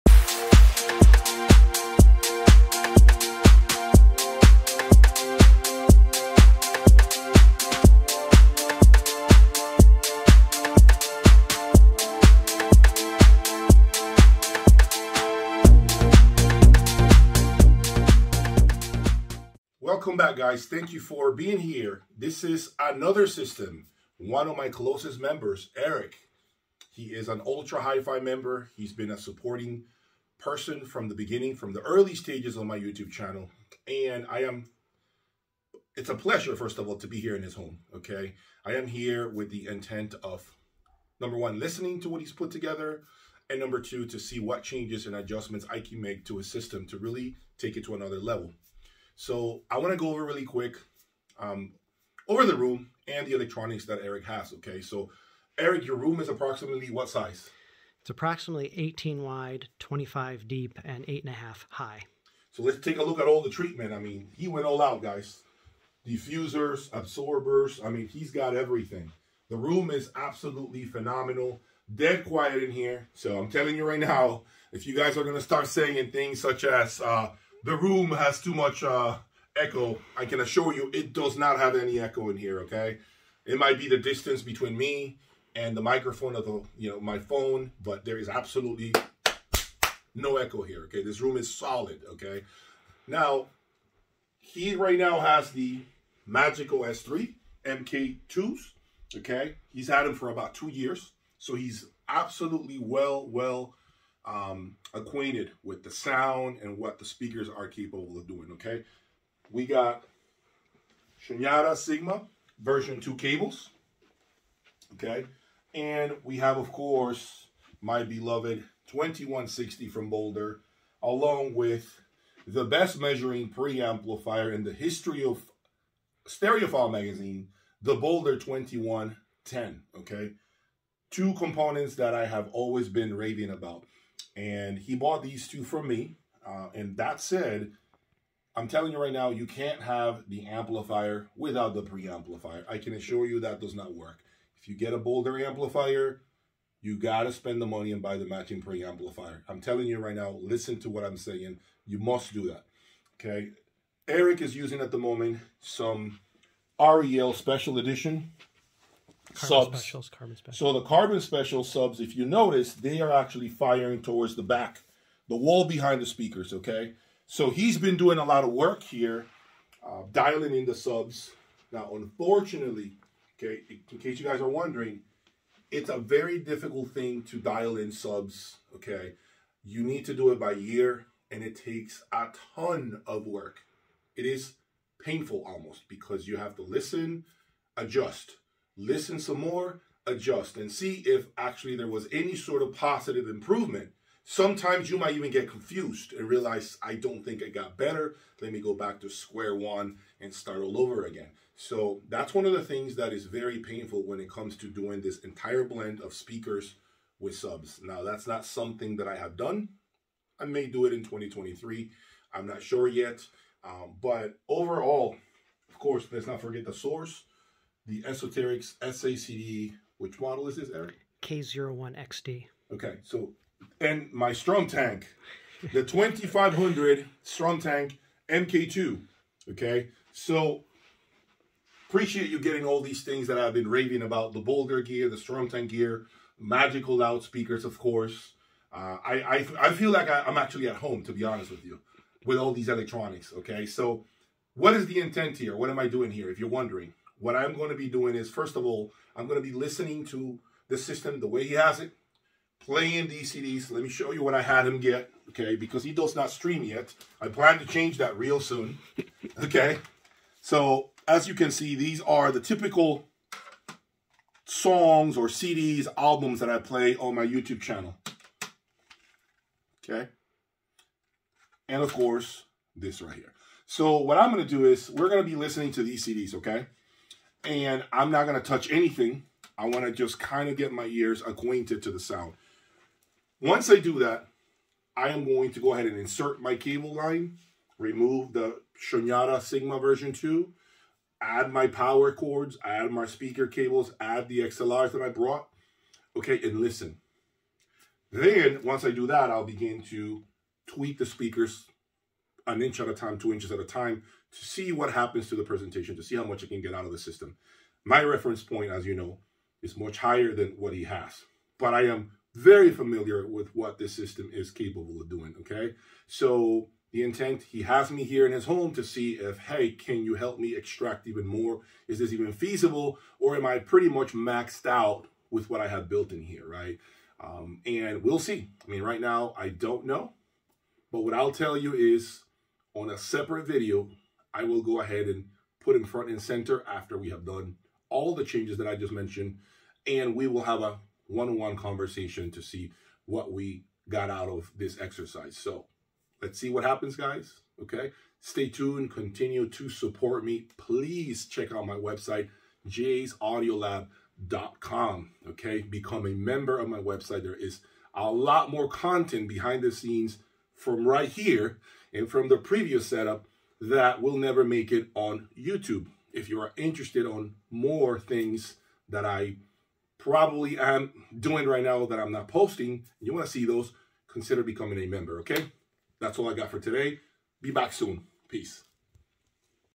Jay's Audio Lab Tweaks Another ULTRA HIFI System!
My new cable line that I feel is the best price performance.

For more exclusive content, consider becoming a member of my website and find out!

 @Jay's iyagi
 @Steve Guttenberg Audiophiliac